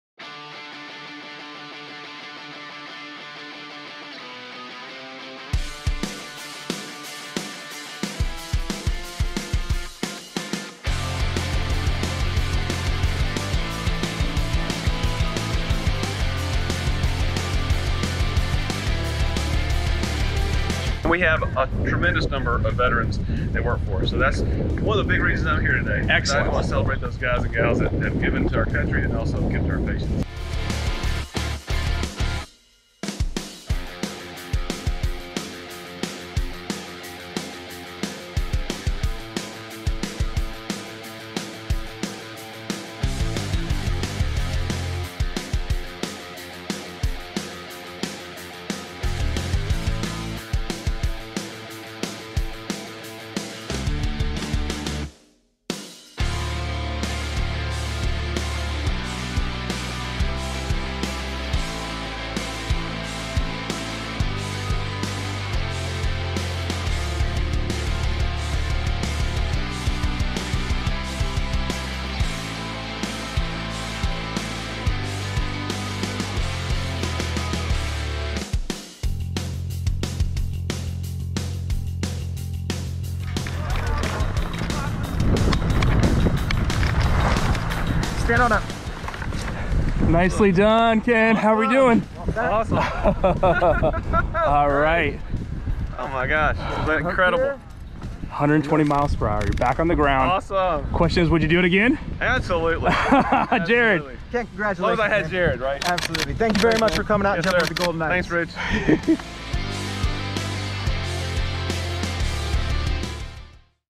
Ken Haynes Skydives with the U.S. Army's Golden Knights
Atrium Health was honored by Forbes as a Best Employer for Veterans. Ken Haynes, enterprise EVP and president of Atrium Health Greater Charlotte, took to the skies in a tandem-skydive with the US Army Golden Knights, who are known as the world’s most elite parachute team, in recognition of Atrium Health’s support of veterans.

As the executive sponsor for Atrium Health’s military veteran system resource group, Ken has been a longtime supporter of our military and veterans over the years – specifically our commitment to Veterans Bridge Home. As part of our military veterans affairs strategy, we are proud of our partnership with Veterans Bridge Home, which connects veterans and their families to organizations like Atrium Health as they transition to become successful and thriving leaders in our community.